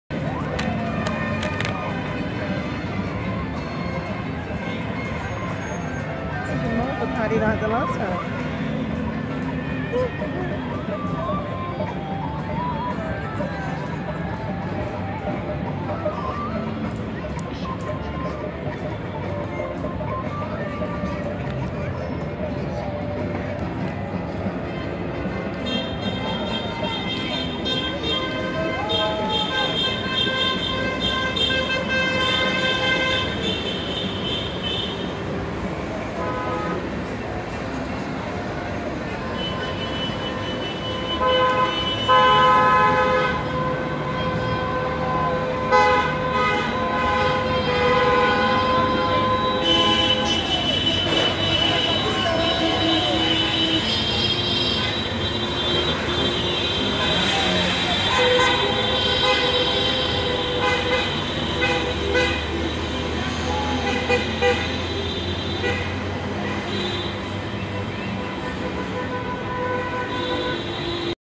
World Cup 2018 Final - After pt 2
Hours after the match along the Seinne. Paris will celebrate long into the night.